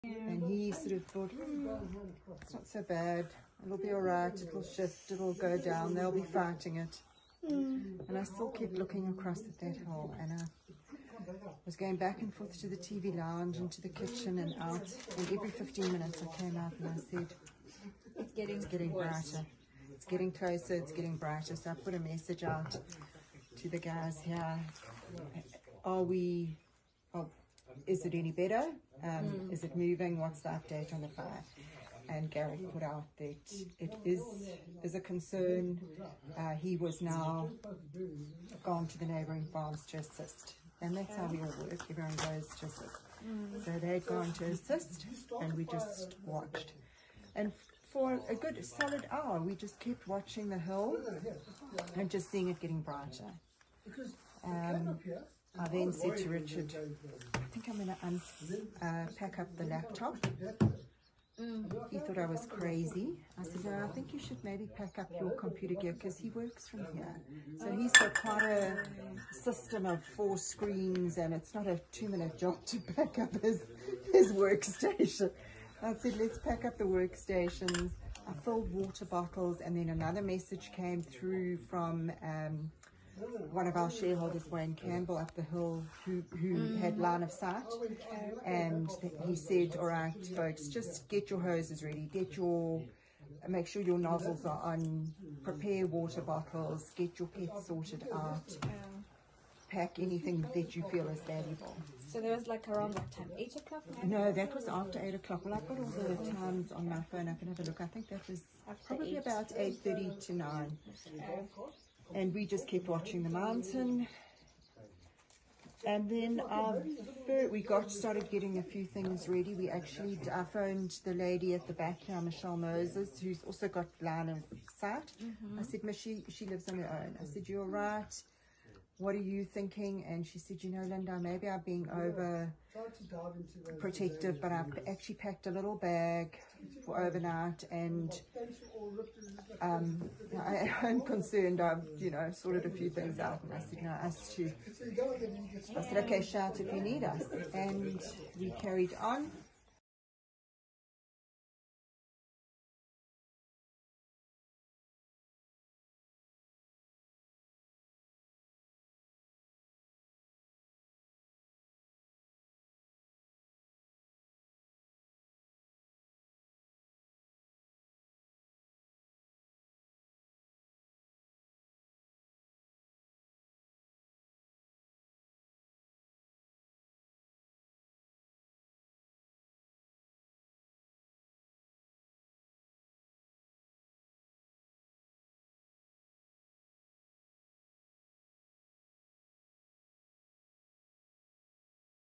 Linda Gorlei, Mbona Nature Reserve resident, talks about how wild fires engulfed the Karkloof areas
Linda Gorlei, who lives in Mbona where two houses, one opposite hers, burnt down on Sunday evening, said the speed at which the fire was moving towards her was fast and frightening.
Video by Londiwe Xulu and Nqubeko Mbhele